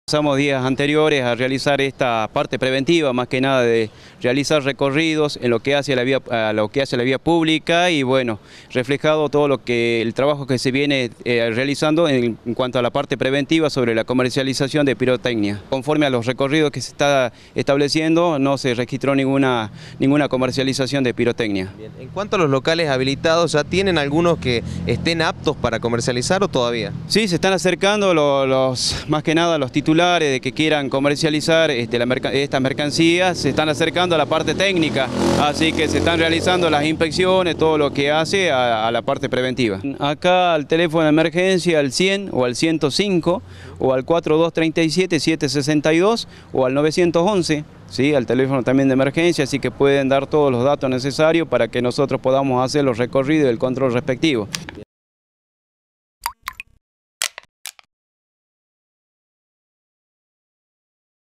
Controles por venta de pirotecnia en locales comerciales
Está prohibida la comercialización de cohetes en las calles de la capital.